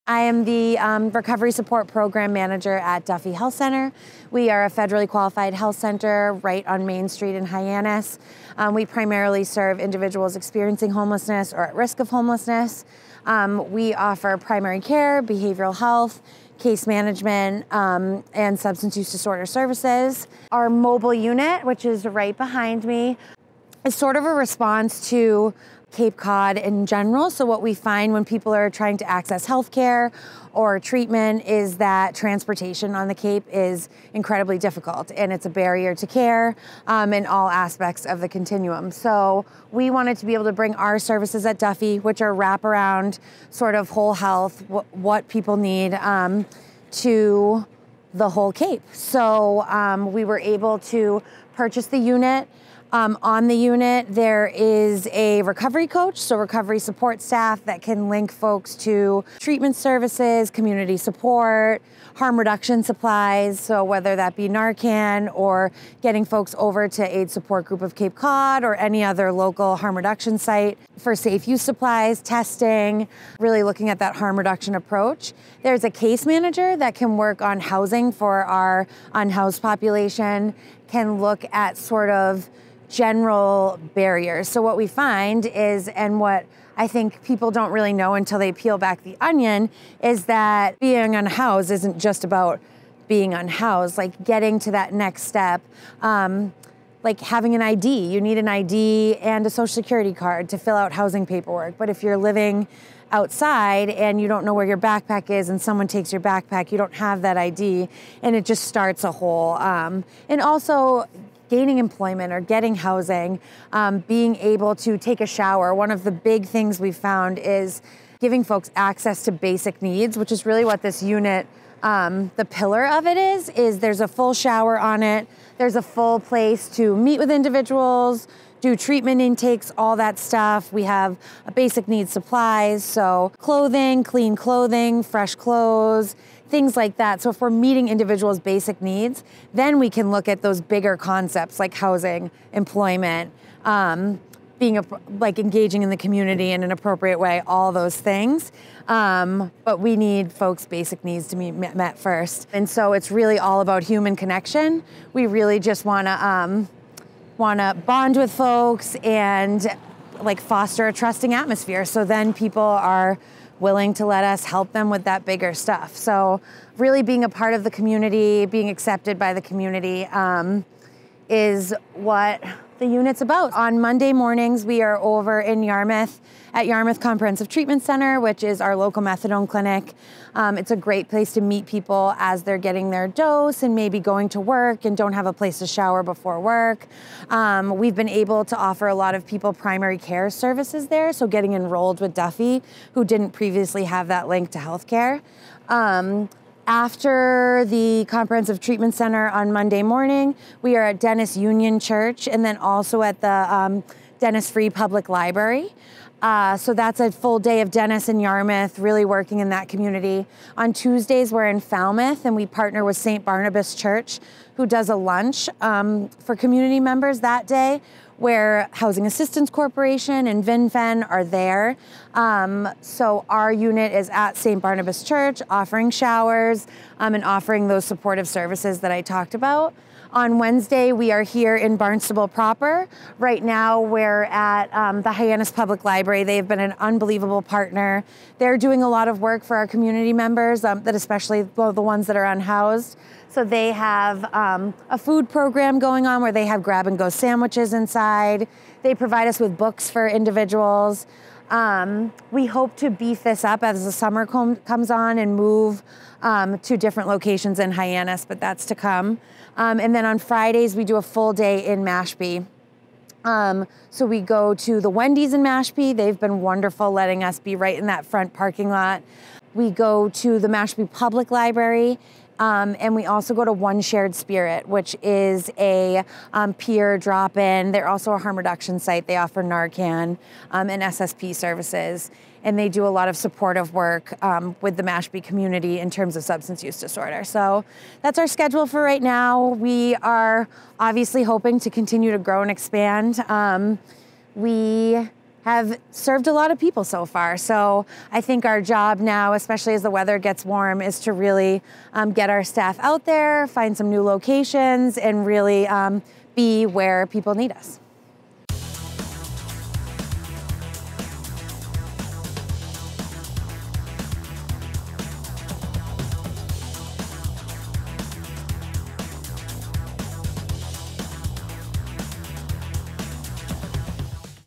An Inside Look At Duffy Health Center's Mobile Unit
Watch Cape Media News on Local Channel 26 every Friday at 5:30pm

Have a news tip or a story to share?